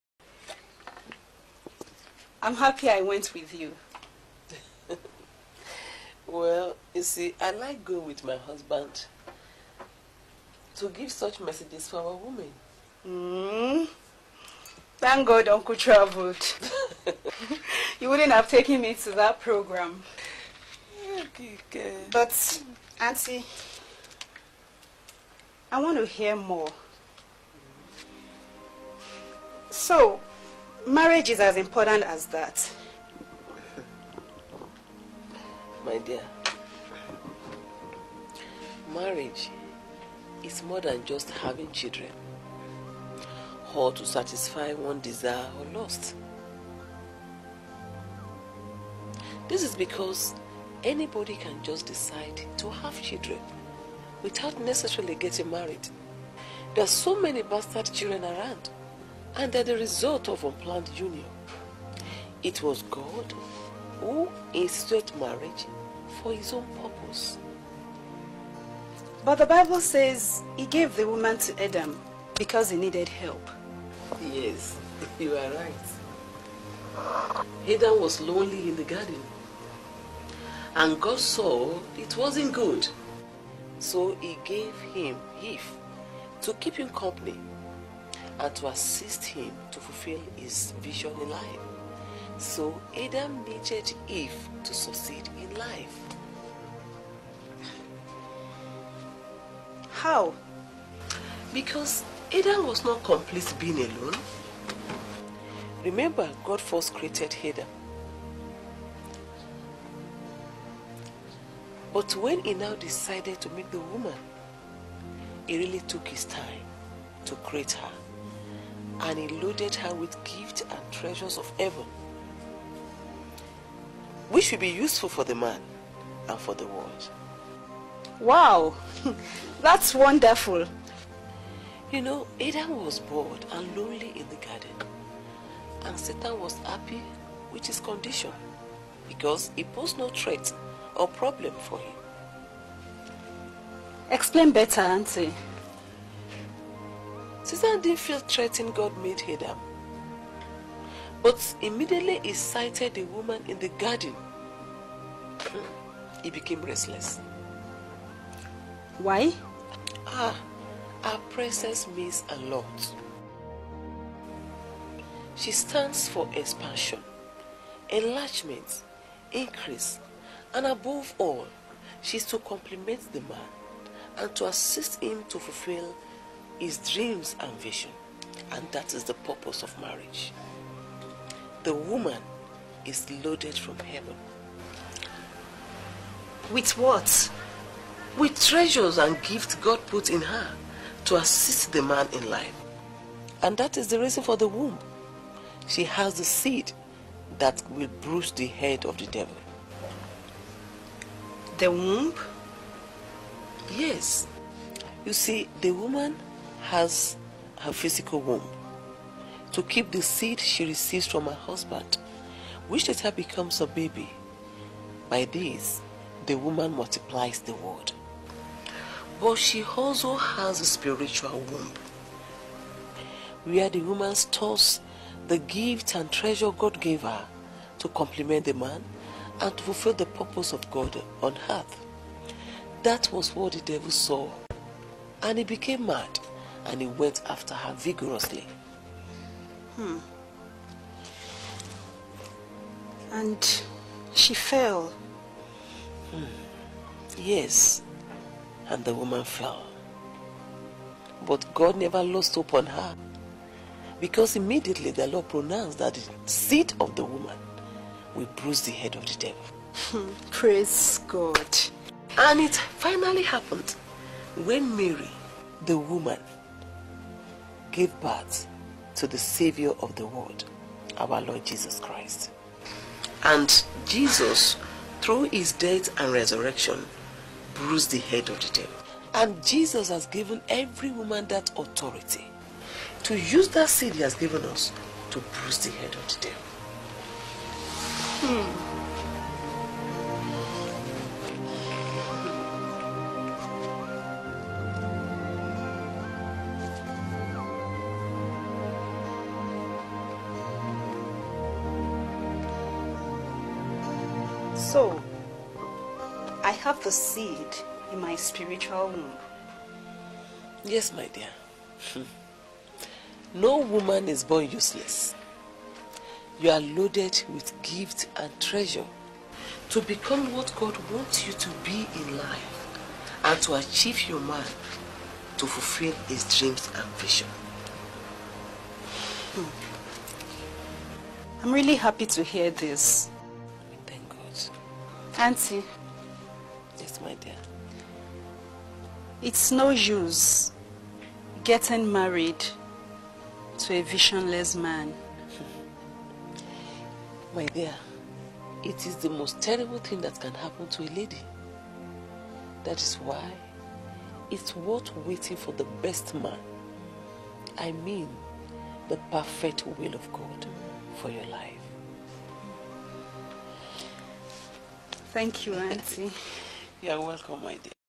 What is Marriage ?||Mount Zion films production
What is Marriage ? How do you understand this Holy Union between man woman( Male and female) as God created it.